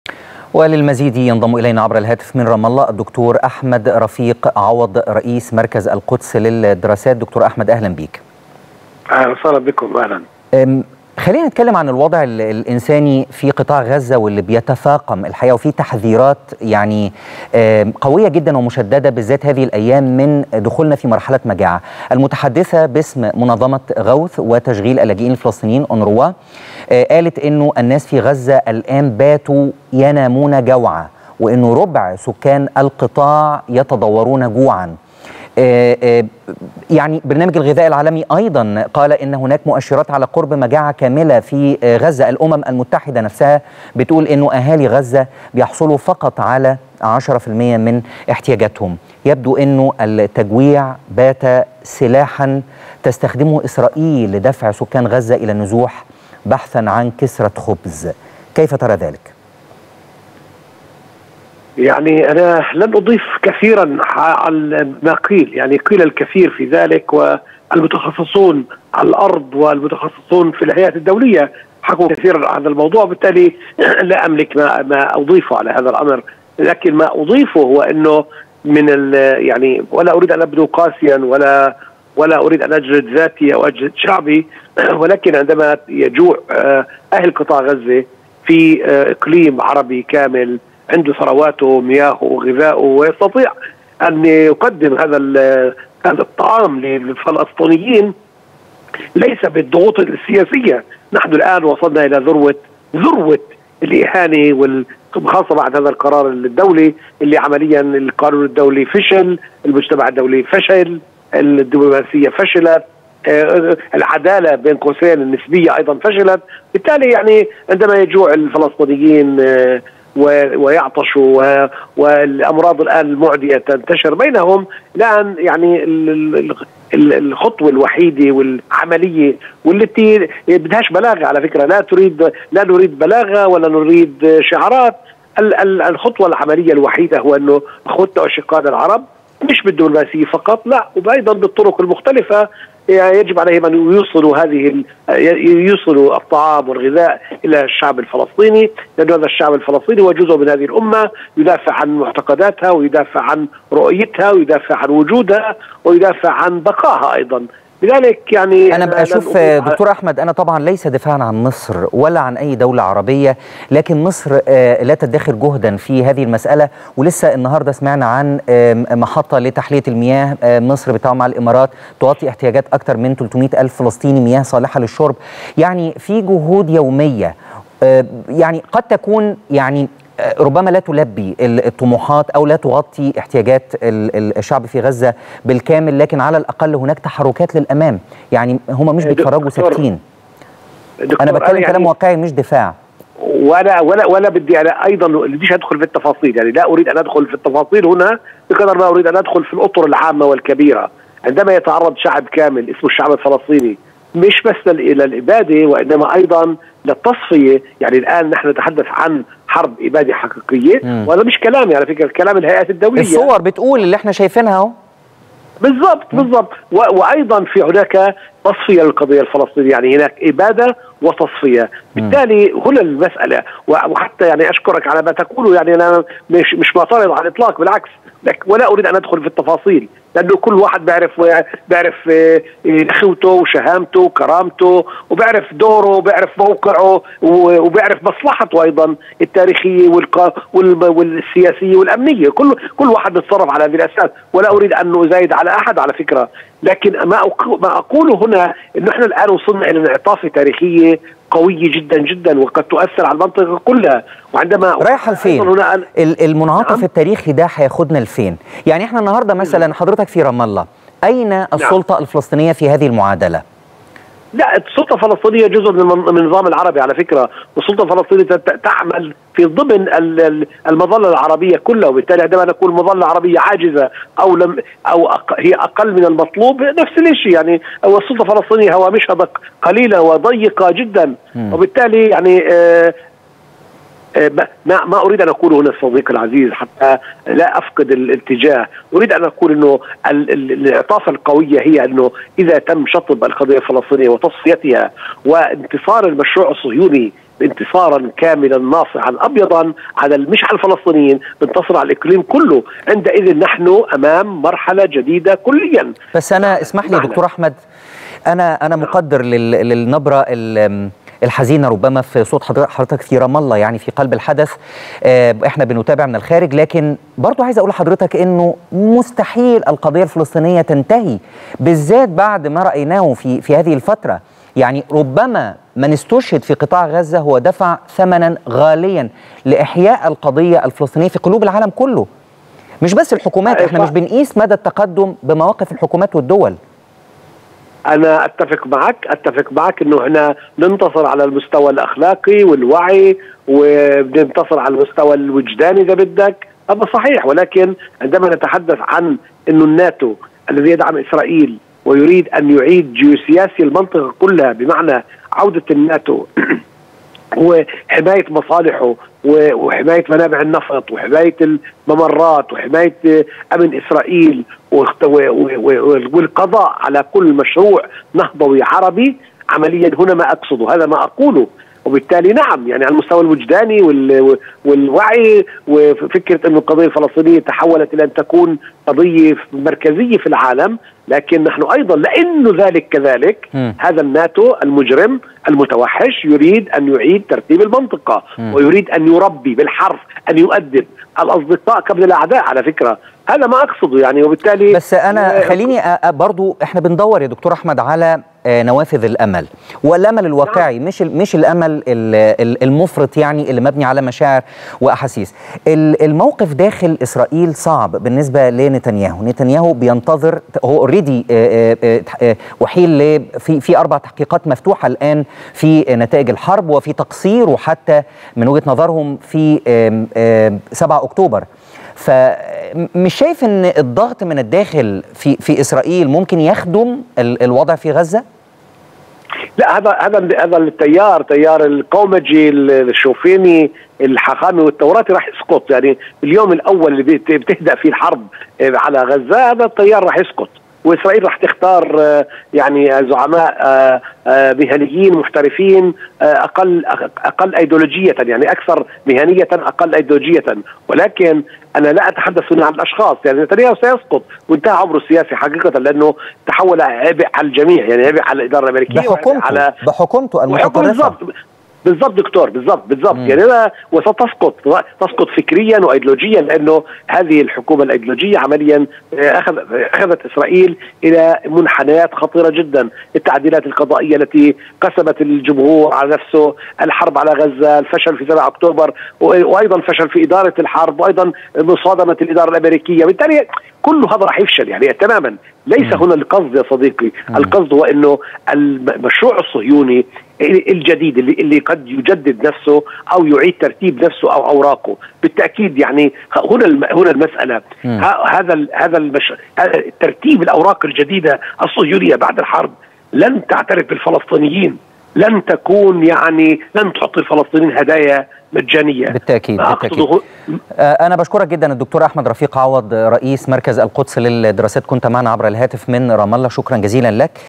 “نحن أمام مرحلة جديدة كليا”.. د. أحمد رفيق عوض: هناك إبادة وتصفية للقضية الفلسطينية
تغطية خاصة| “نحن أمام مرحلة جديدة كليا”.. د. أحمد رفيق عوض رئيس مركز القدس للدراسات: هناك إبادة وتصفية للقضية الفلسطينية